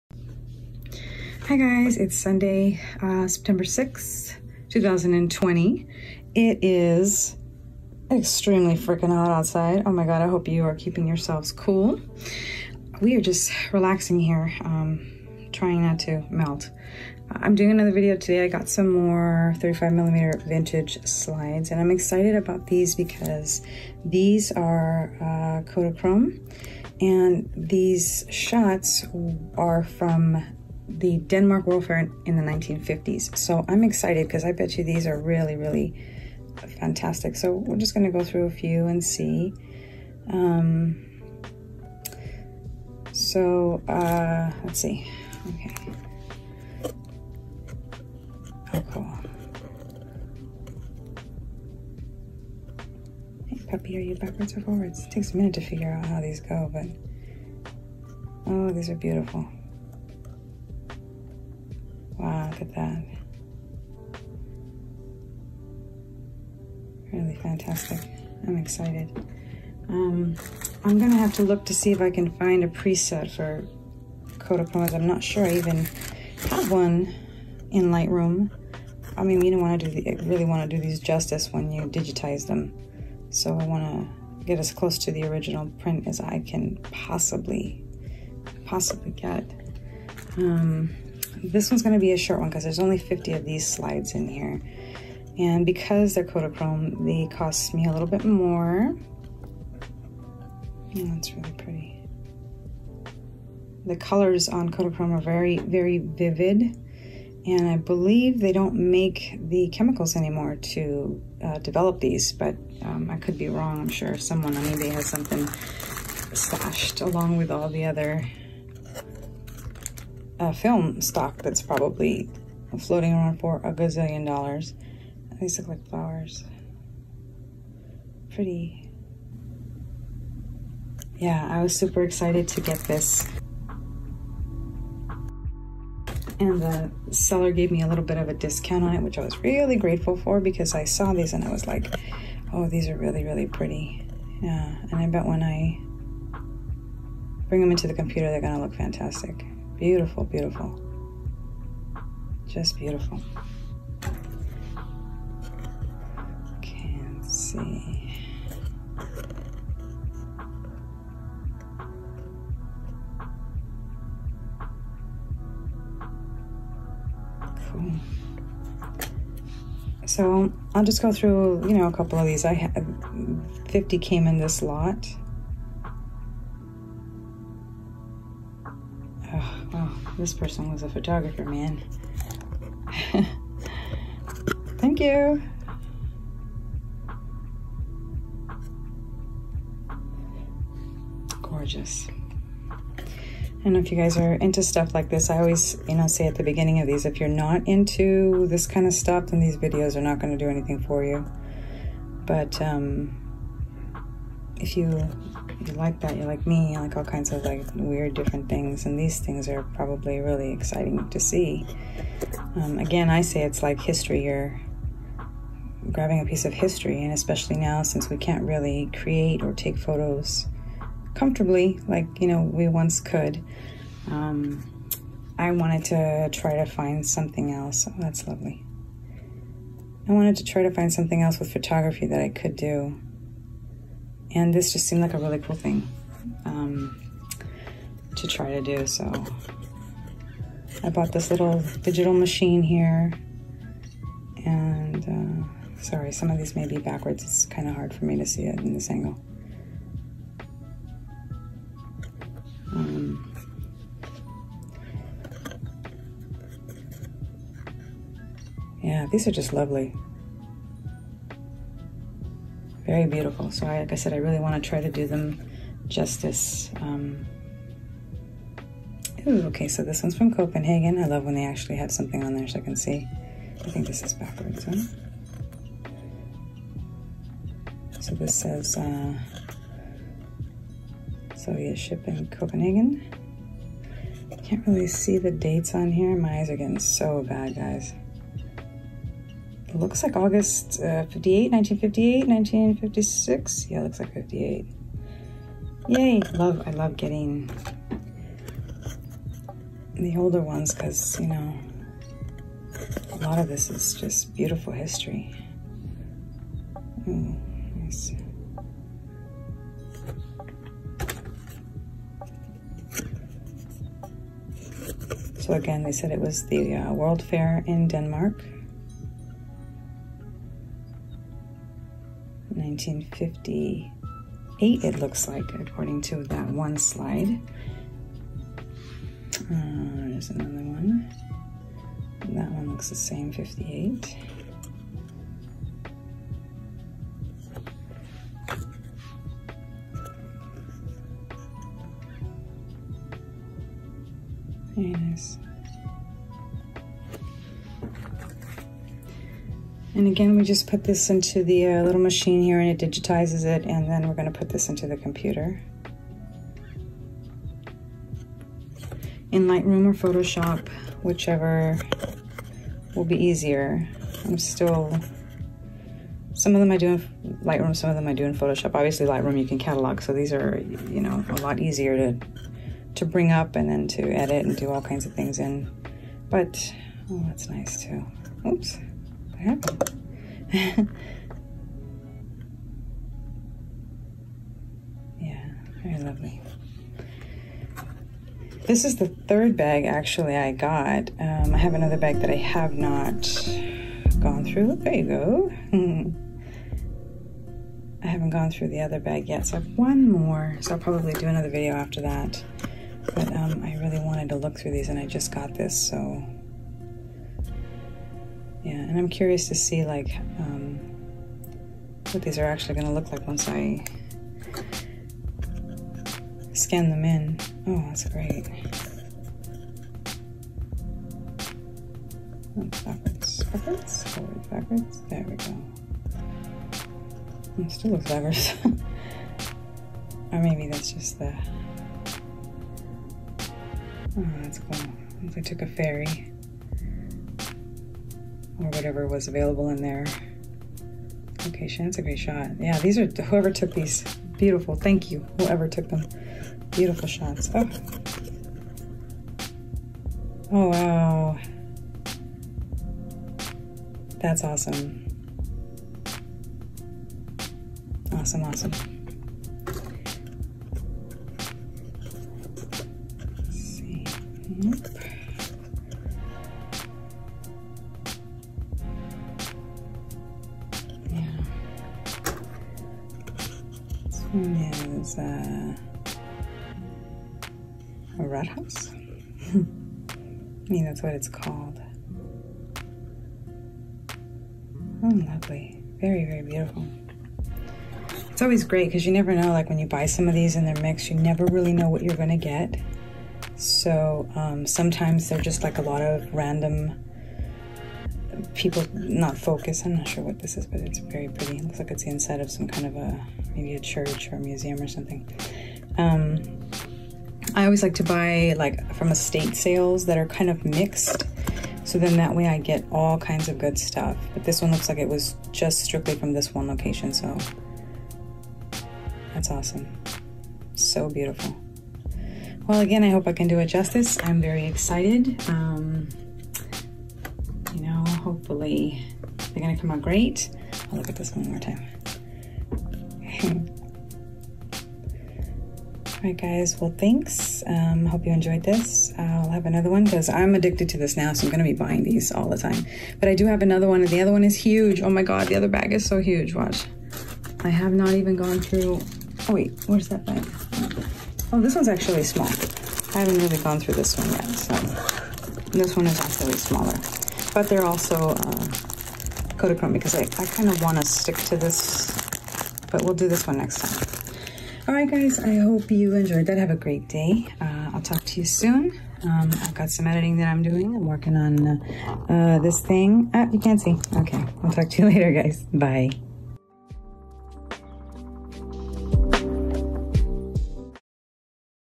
Vintage Found Slides, Going through my eBay haul Episode 2
Here is a look through the lot of Vintage 35mm Found Positive Slides I bought from Ebay.  The first episode is on my IG TV Follow me @Starrchez

If you're into this kind of thing you will enjoy the find.

Hope you Enjoy it. More episode to follow.